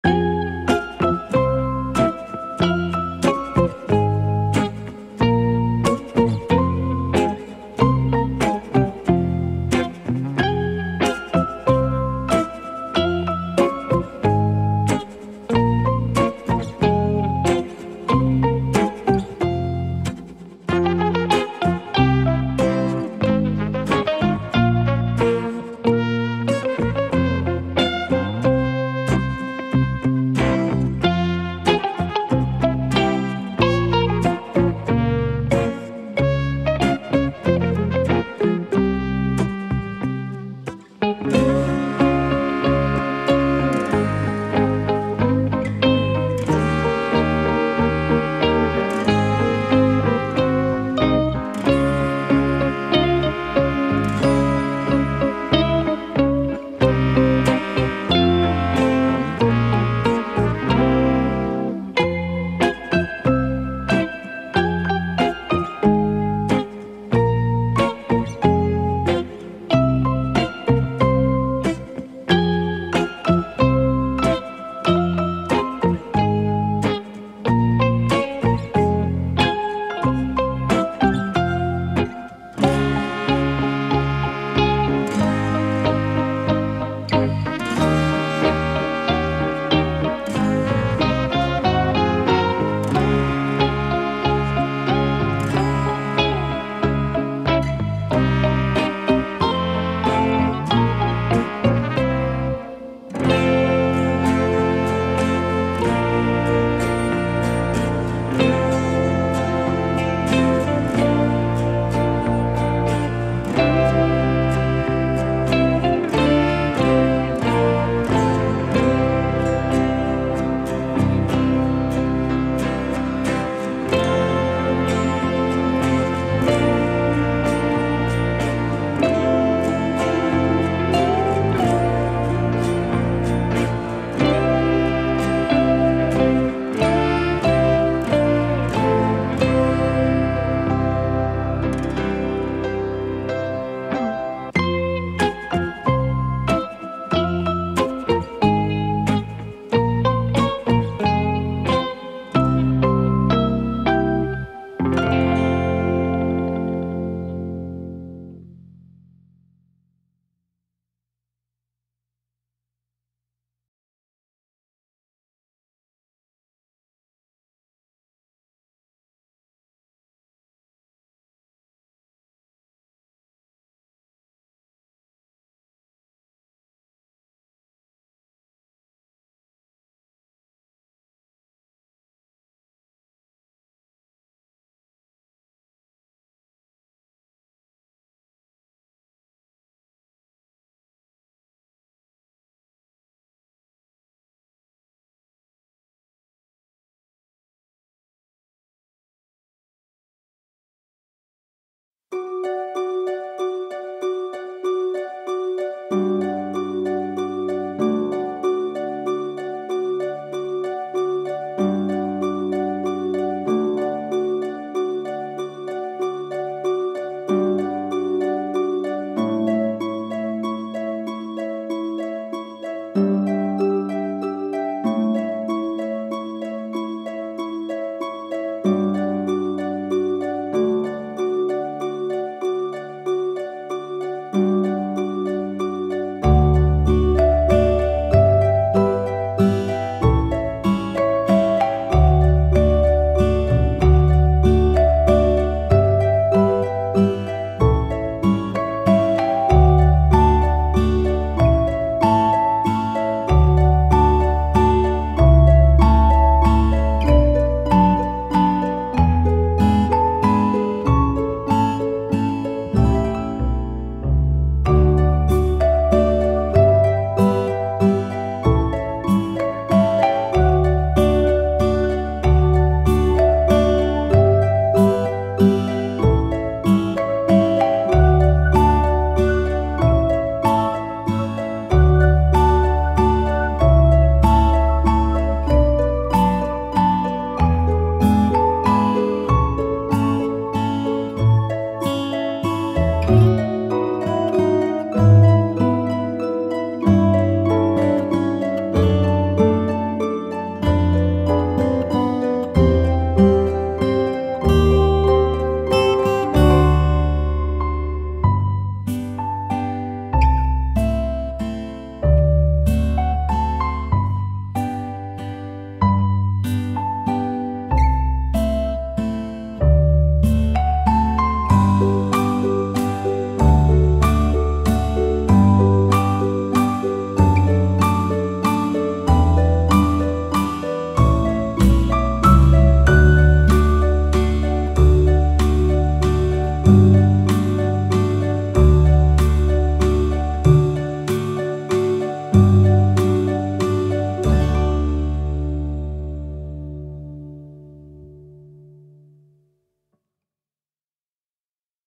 Coloring AMY with colored pencils | AMY Coloring Pages | SONIC HEDGEHONG
Welcome to Simplynata Channel.
Today we are going to coloring Amy.